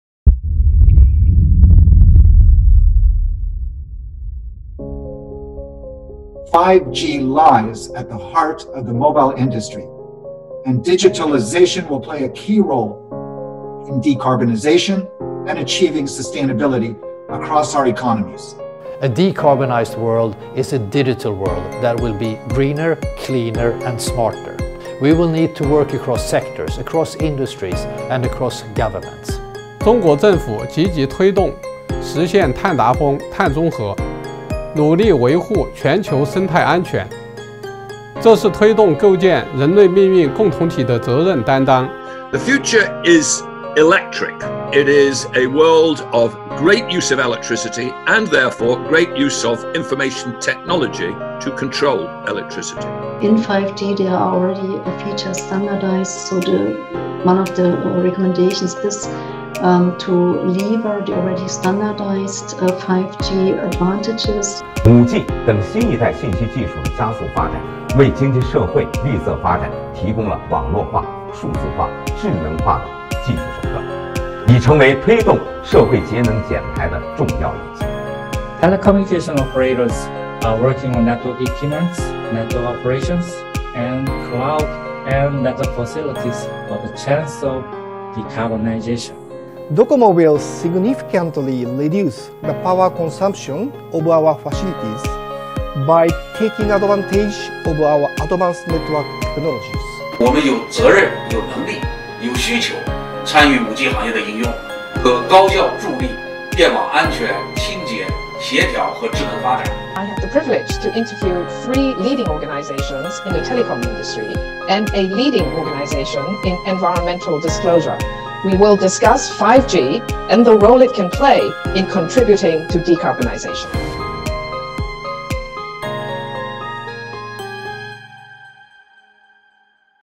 A deep dive into 5G and Decarbonization!——GTI Online Summit 2022
A deep dive into 5G and Decarbonization! GTI Online Summit 2022 will gather TOP line-up of speakers from organizations,operators, vendors and verticals to discuss the current status and strategies of 5G development, and how technology innovation will play a role in reaching the goal of carbon neutral and net zero.